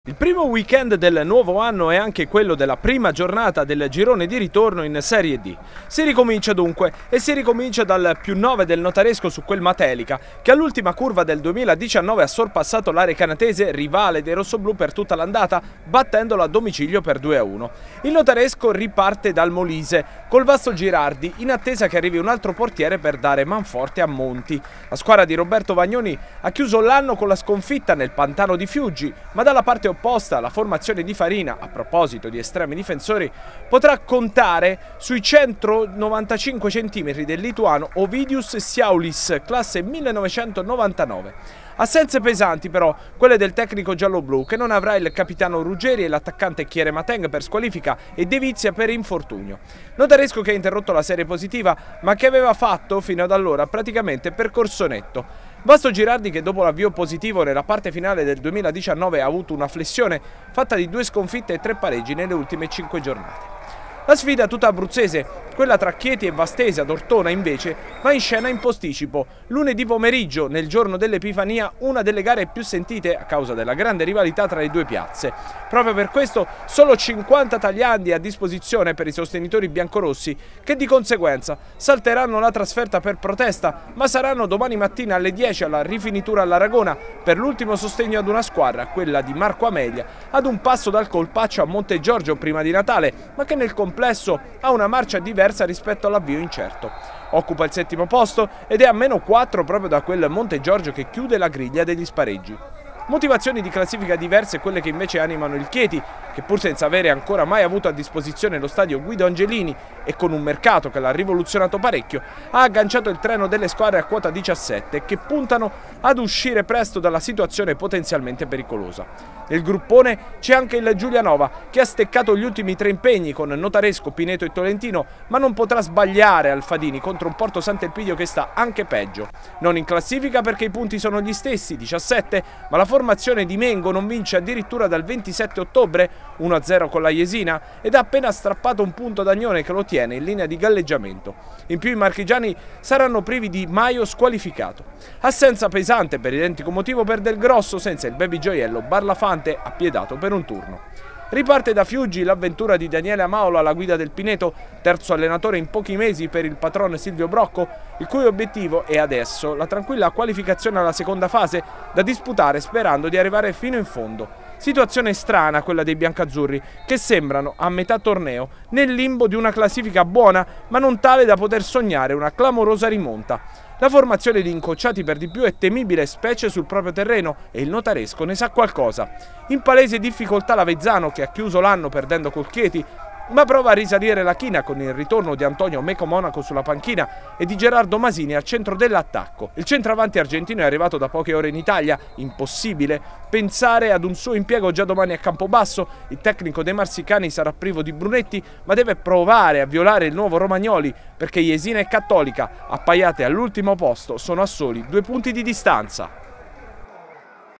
La Serie D apre il 2020 del calcio
Primo fine settimana del 2020 con la Serie D che torna per la prima del girone di ritorno. Il Notaresco guida la classifica mentre in basso tre abruzzesi devono fare punti. Lunedì pomeriggio, giorno dell'Epifania, scontro tutto abruzzese ad Ortona tra Chieti e Vastese